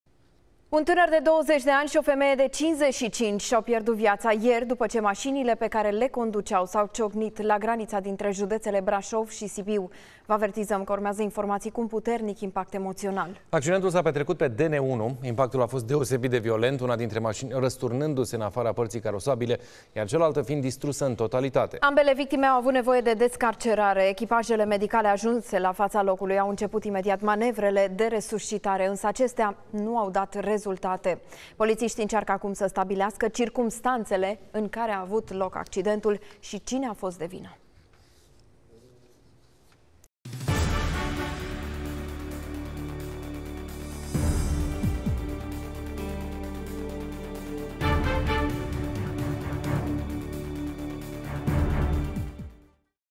Accident înfiorător pe DN1, între Brașov și Sibiu. O femeie și un tânăr de 20 de ani au murit
O femeie și un tânăr de 20 de ani au murit într-un accident produs luni după-amiază pe DN1, la granița județelor Brașov și Sibiu.